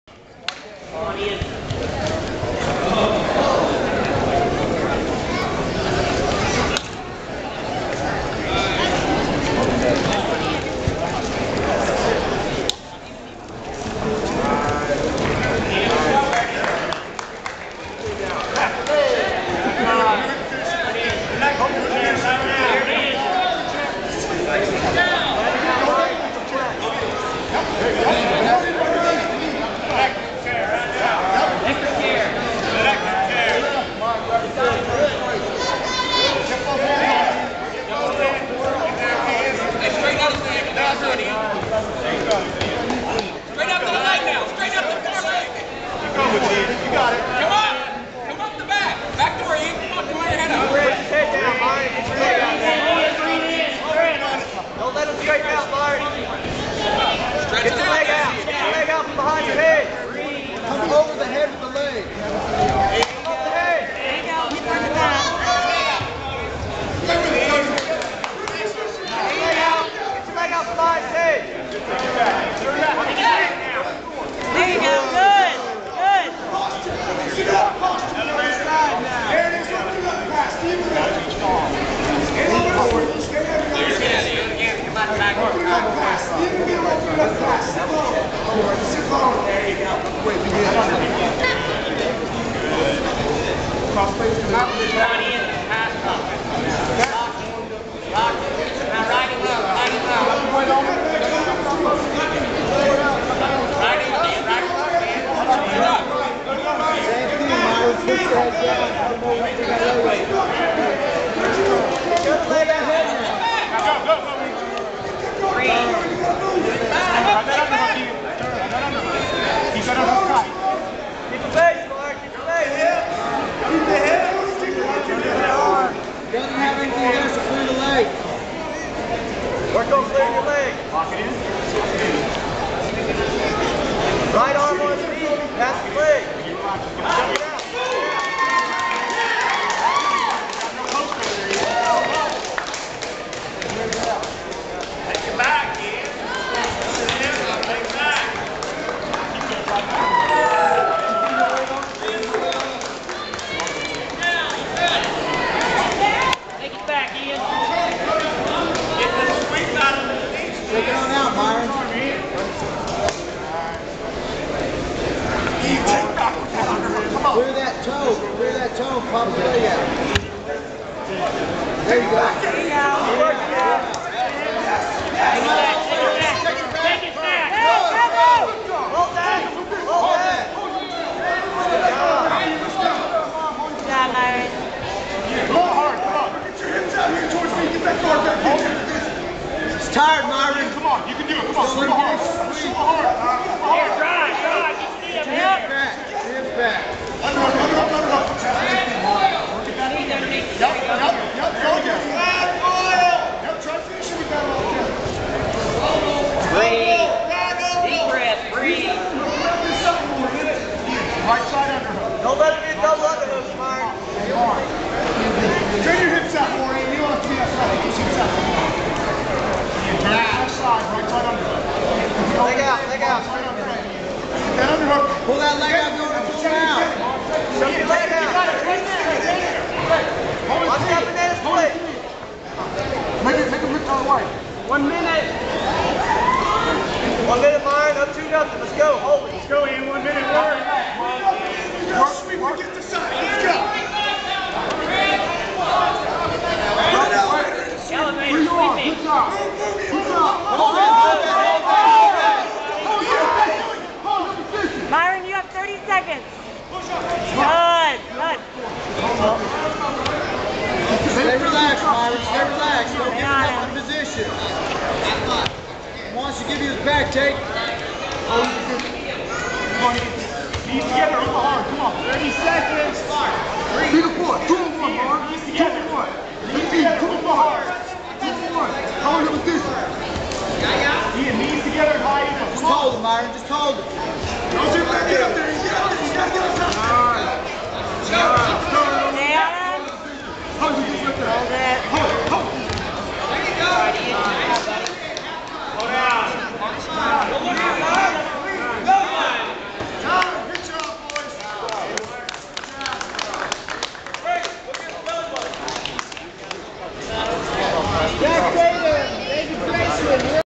(AMA) Myron round 2 at 4th Annual Mikey Burnett Submission Grappling Tournament
4th Annual Mikey Burnett

Submission Grappling Tournament

Edison High School

2906 E. 41st